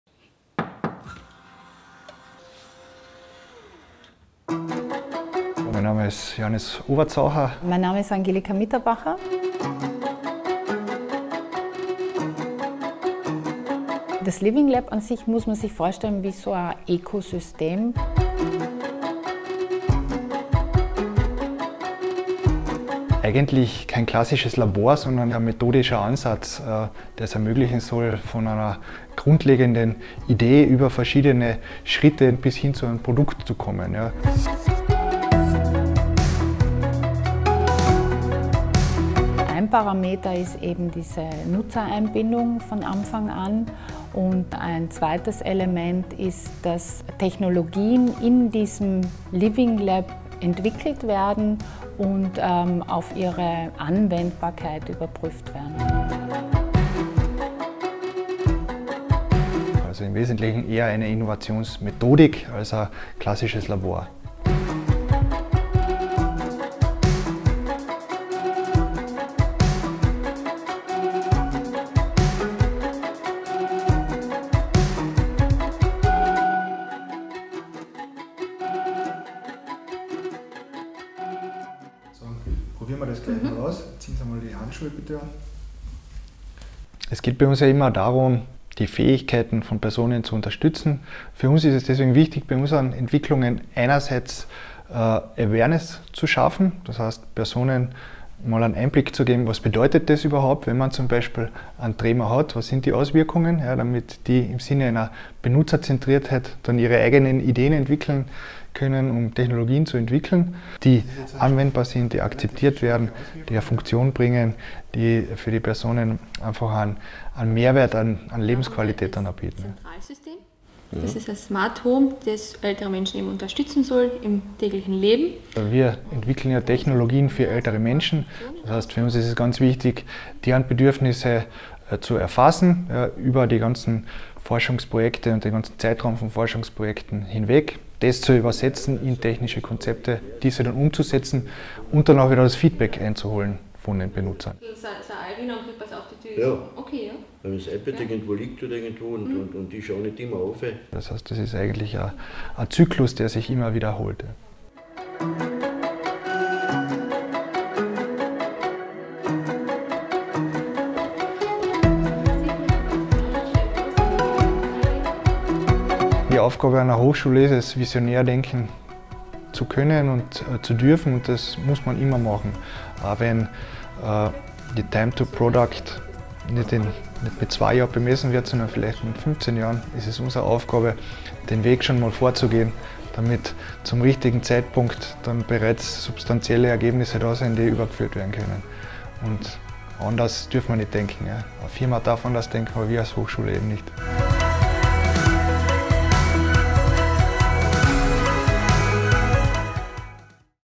Medizintechnik im Living Lab
Viele Senioren wollen in den eigenen vier Wänden alt werden. Damit das gelingt, wird in der Zielgruppe nach assistiven Lösungen geforscht. Im Living Lab Carinthia der FH Kärnten forschen Lehrende und Studierende nach benutzerzentrierten Methoden, die auf ihre Anwendbarkeit am Menschen geprüft werden.